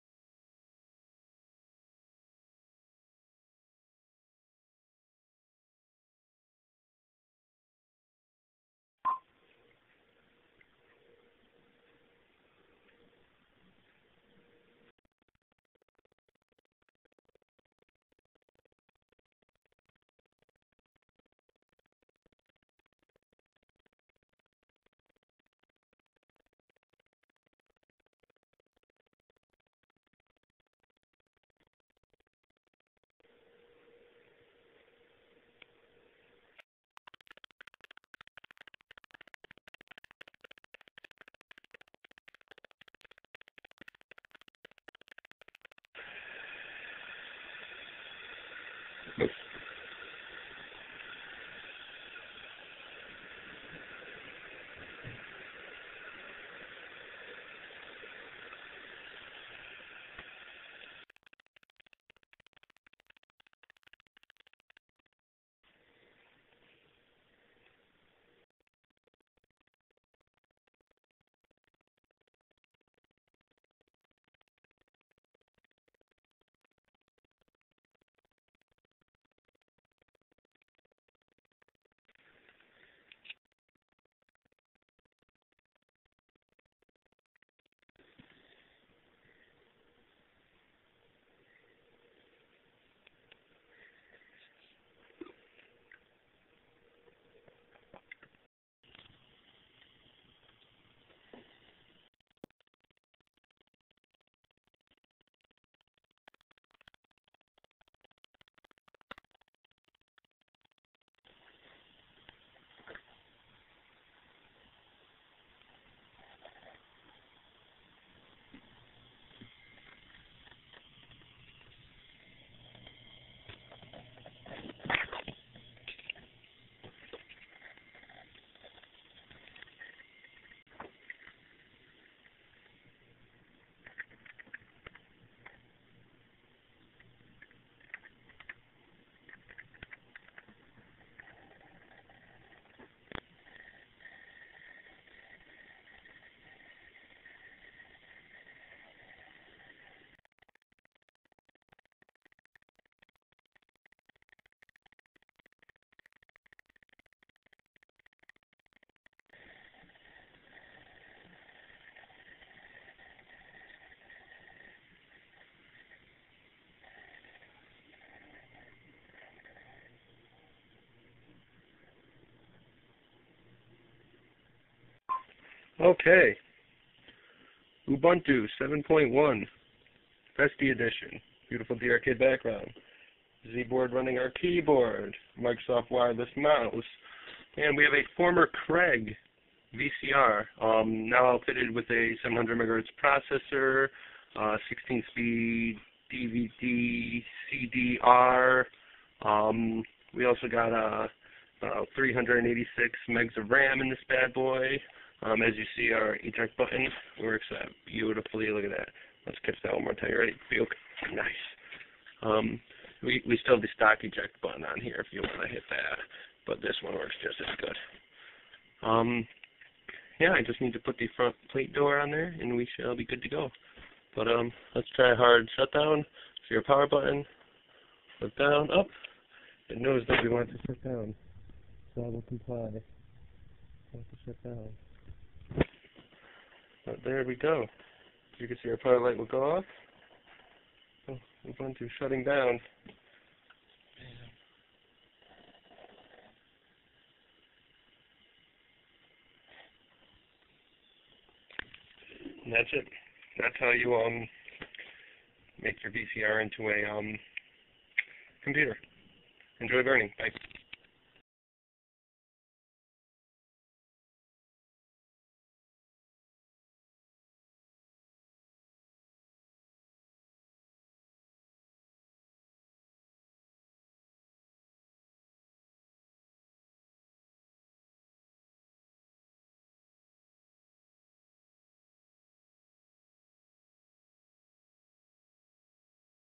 VCR CPU
pent 3 cpu installed fully in old ass VCR